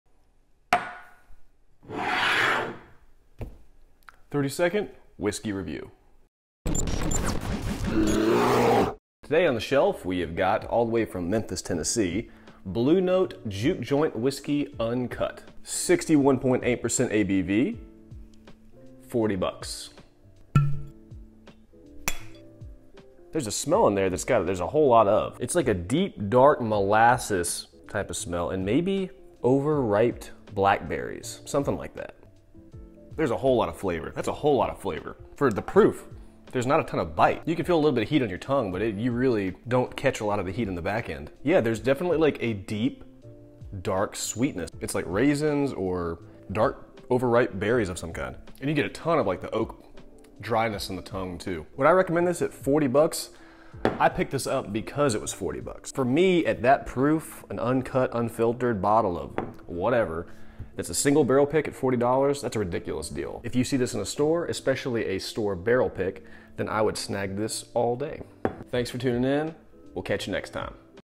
Blue Note Bourbon Uncut/Unfiltered - 30 Second Whisk(e)y Review
A heavy hitter from Memphis, TN.

On tap today: Blue Note Bourbon Uncut/Unfiltered

BOTTLES FEATURED:
-Blue Note Bourbon Uncut/Unfiltered